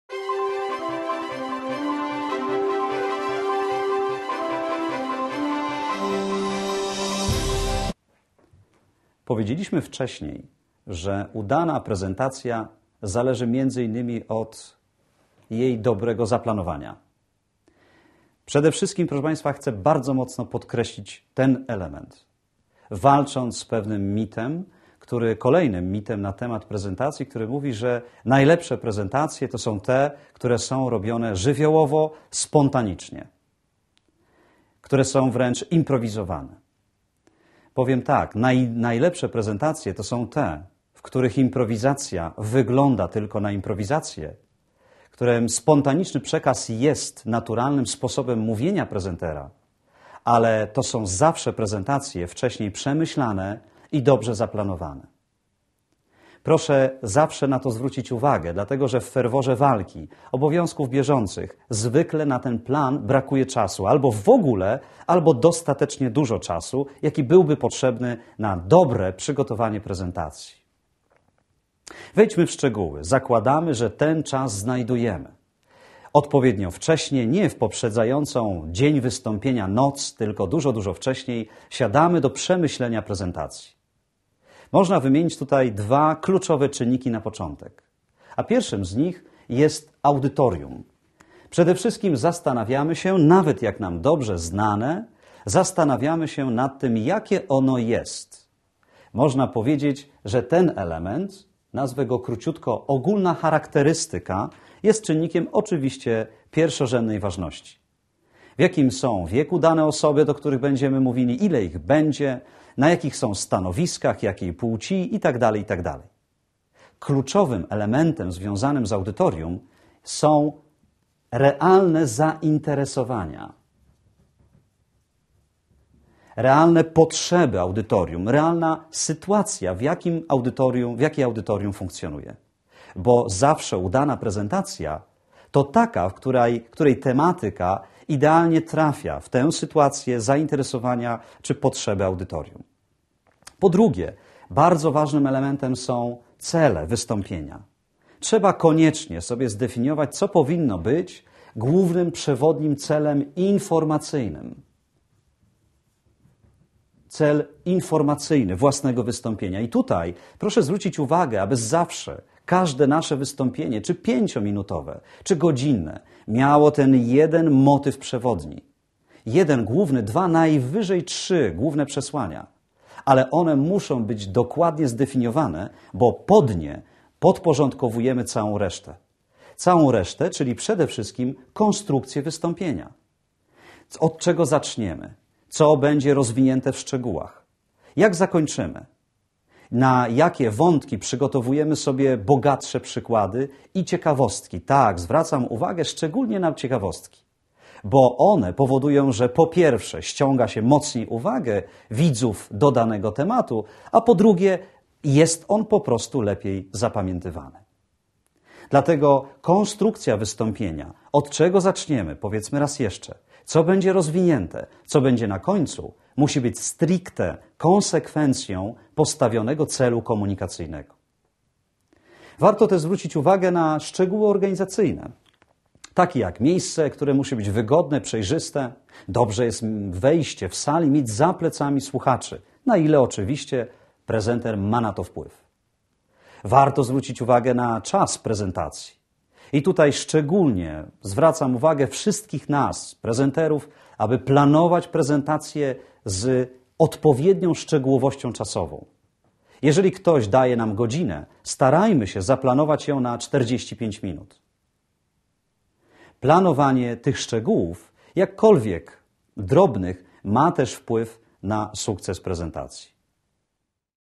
Plan prezentacji
Najlepsze prezentacje to te, których improwizacja wygląda jedynie na improwizację, a spontaniczny przekaz jest naturalnym sposobem mówienia prezentera, ale są to zawsze prezentacje wcześniej przemyślane i dobrze zaplanowane. Jak najlepiej się przygotować?